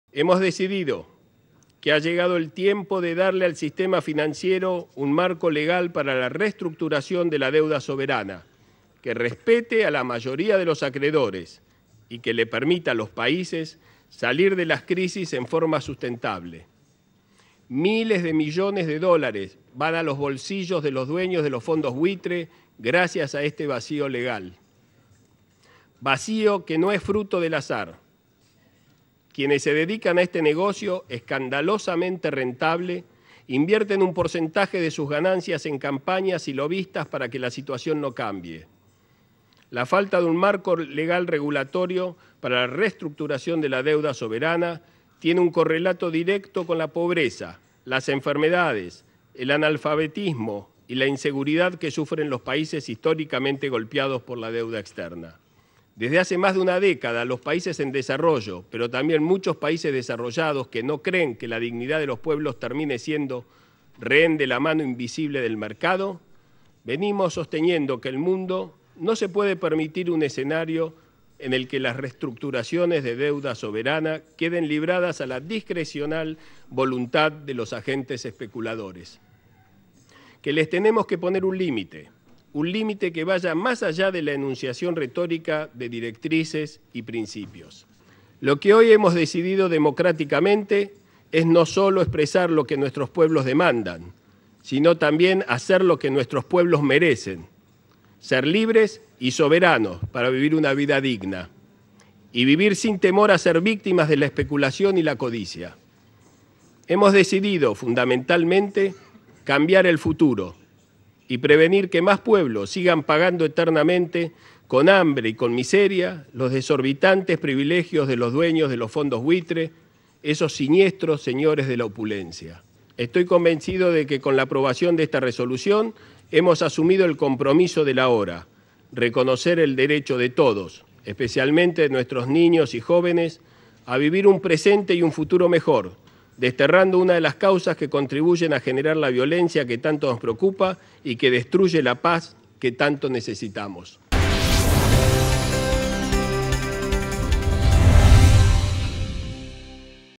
Timerman: votación en la ONU de un marco jurídico para reestructuración de deuda soberana
El canciller, Héctor Timerman, dijo hoy que lo que se ha decidido en la ONU “expresa lo que nuestros pueblos demandan y merecen”, en referencia a la aprobación de la discusión sobre un marco jurídico para procesos de reestructuración de deuda soberana.

“Si la ONU fue capaz de regular hasta los aprovechamiento de los recursos marítimos, cómo no podríamos decidir lo que hoy hemos decidido”, se preguntó el canciller, al hacer uso de la palabra luego de la votación en la que la propuesta impulsada por Argentina recibió 124 votos positivos.

"Estoy convencido de que hemos asumido el compromiso de reconocer el derecho de todos a vivir un presente y un futuro mejor, desterrando una de las causas que genera la violencia que tanto nos preocupa”,concluyó